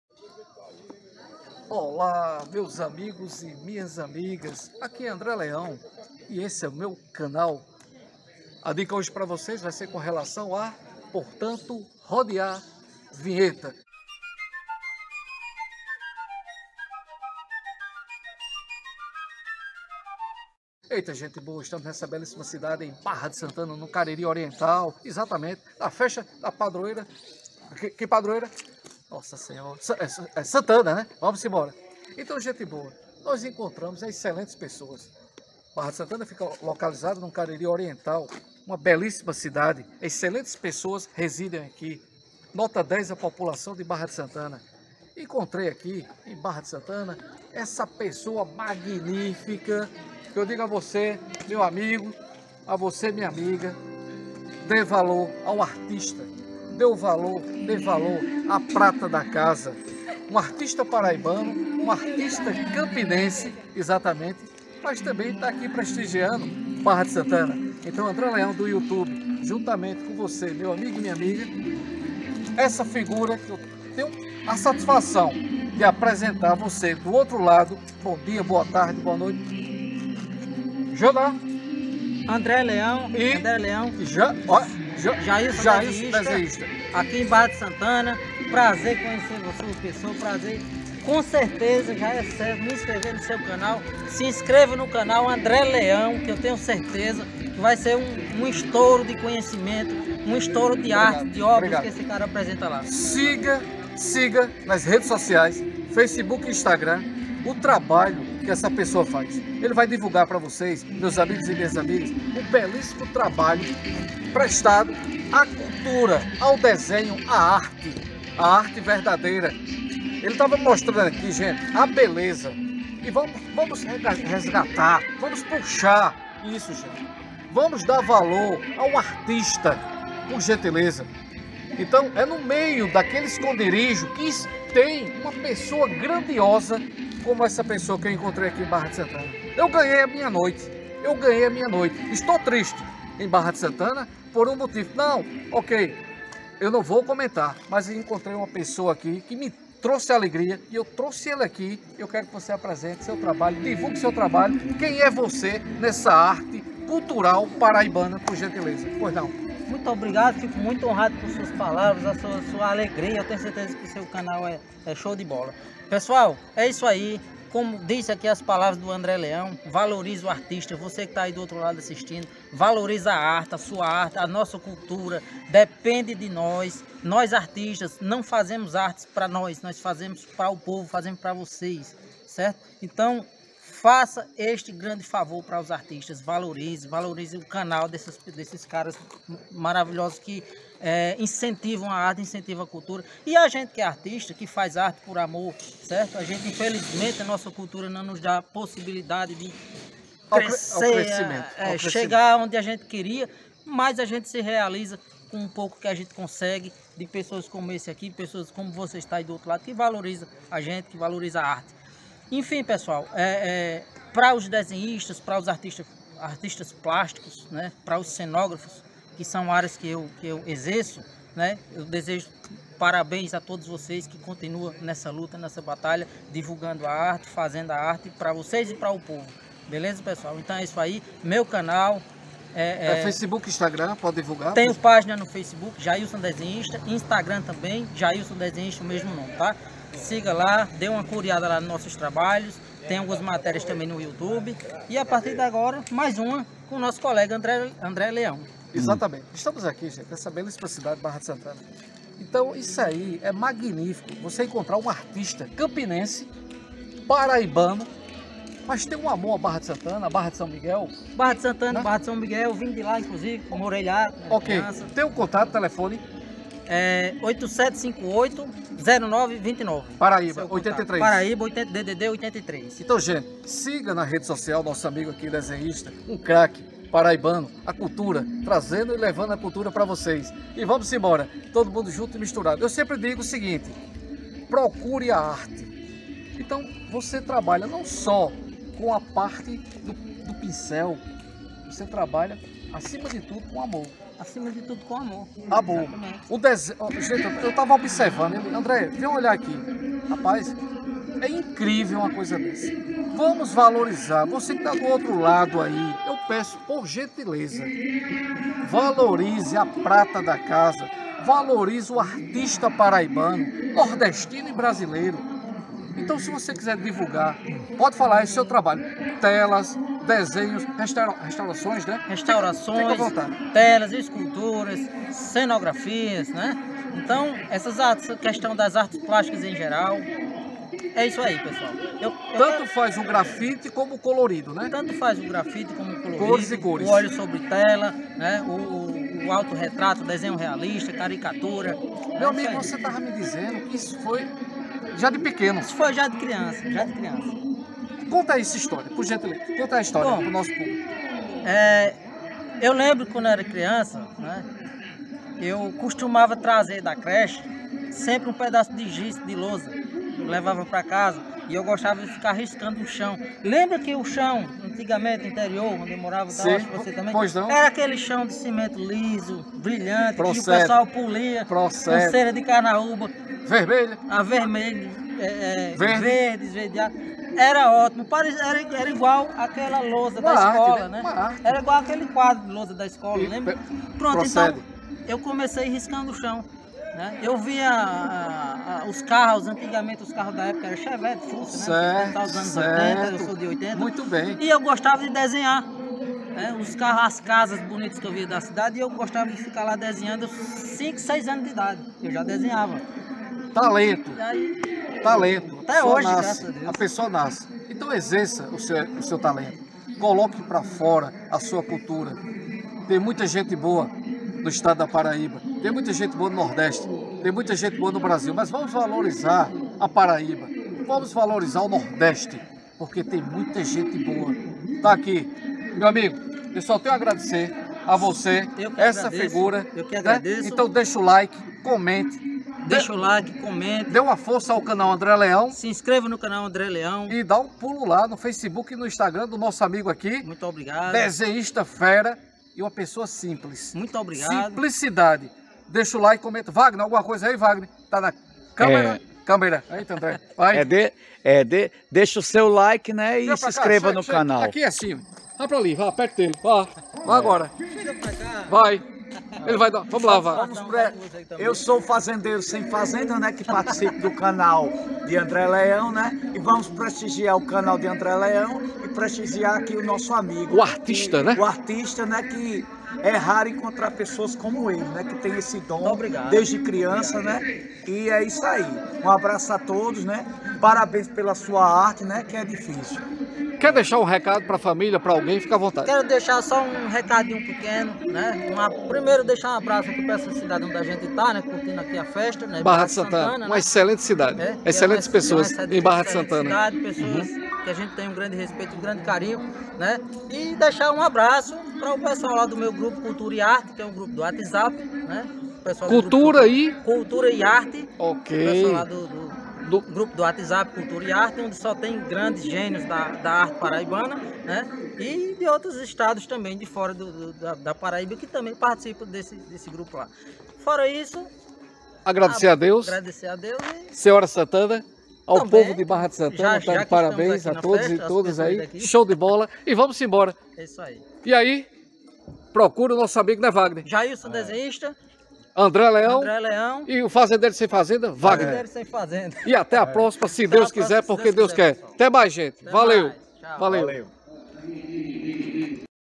O Dom, A Arte e O Artista
Estivemos na festa da Padroeira Senhora Santana em Barra de Santana - PB, e encontramos um Excelentíssimo Desenhista Plástico Paraibano Jailson e fizemos uma entrevista com o mesmo. Todos nós que fazemos parte da equipe André Leão agradecemos a todos vocês.

Equipe:
            André Leão, Luiz Arthur e José Alexandre.